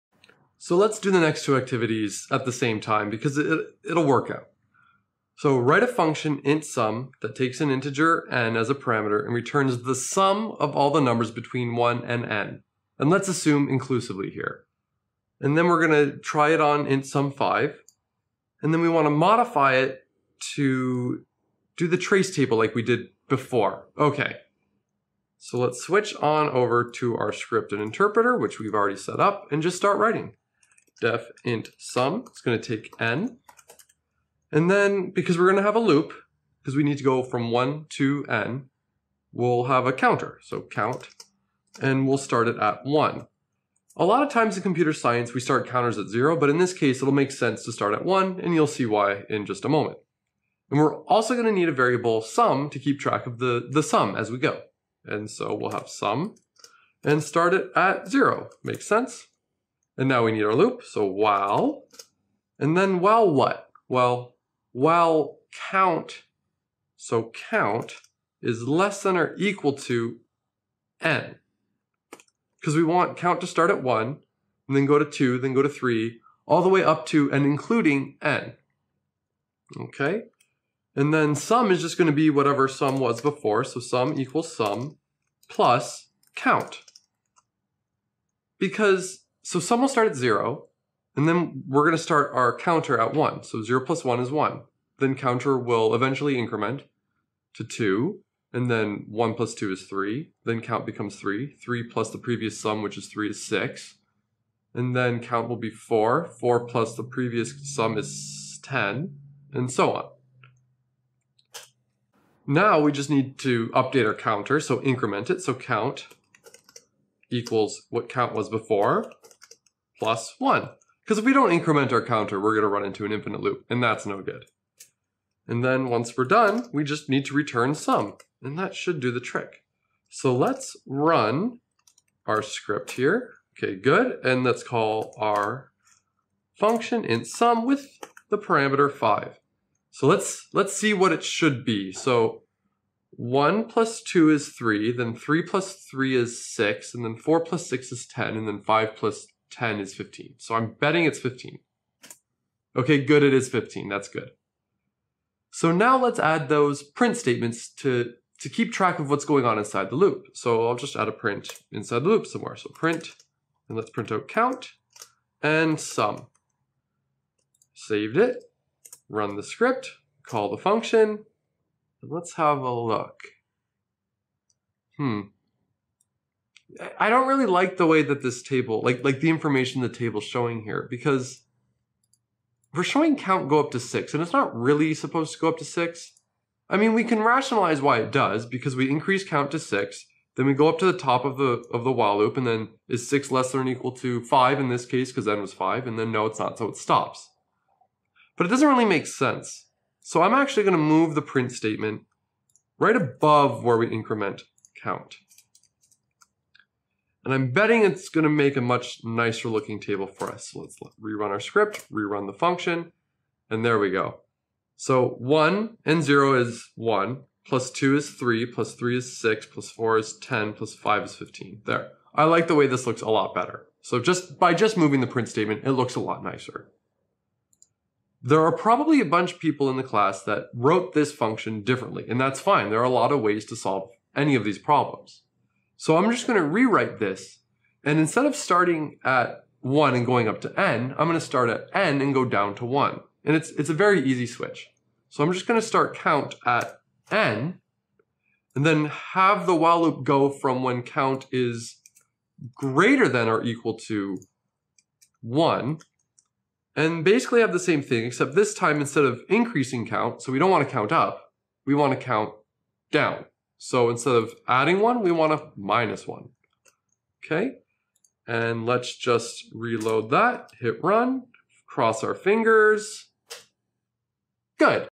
6.2 - intsum
Creating a function to sum all the numbers from one to some arbitrary number (and making sense of it).

VERSION WARNING: If you're not Python 2.7, that's cool. All of the stuff will work the same EXCEPT for the print statement. Just make this change...

change: print something
to: print(something)

In other words, just put brackets around the thing you're trying to print.